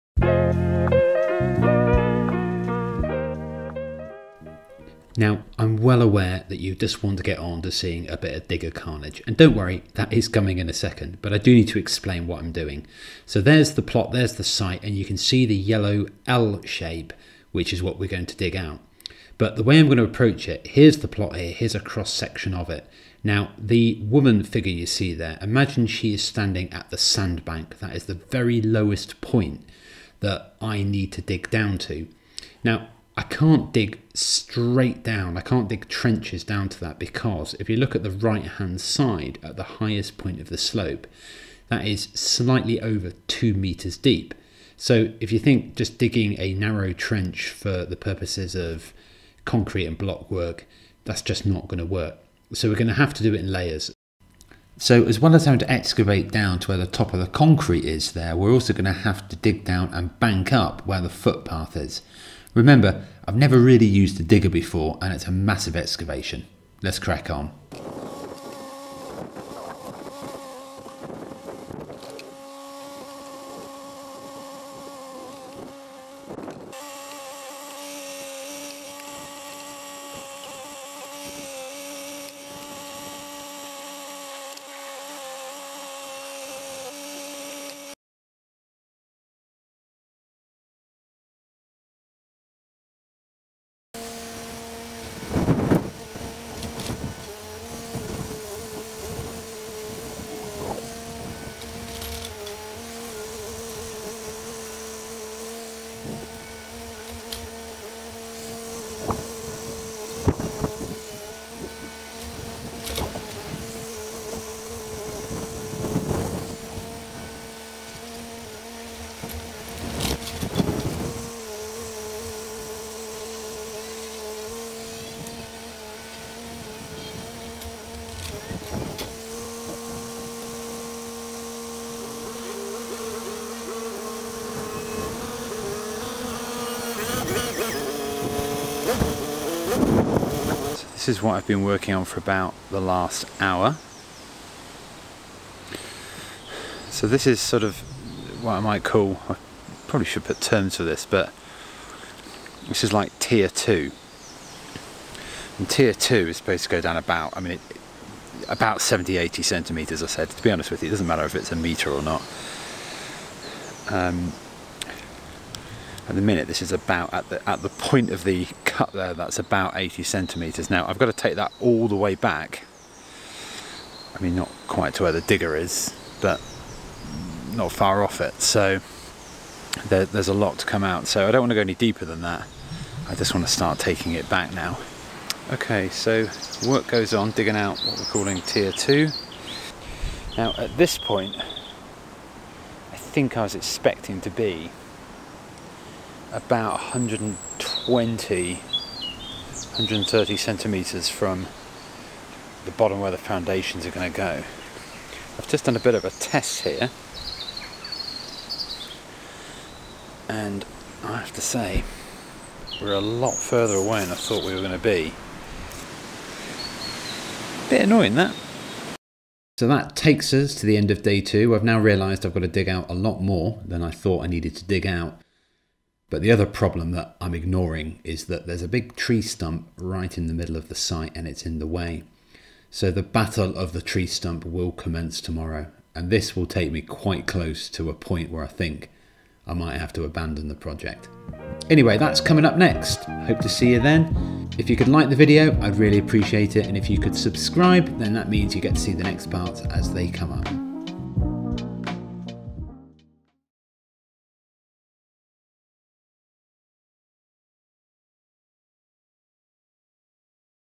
Sloping site deep excavation - DIY Barn Build #4
In episode 4 we excavate over 2 metres down into subsoil across the whole site.

I'm a DIY-er and I'm about to embark on building a 85m2 barn / workshop out of quarried stone & timber, on a 2metre drop slope, with no building experience.

Let's see how I get on...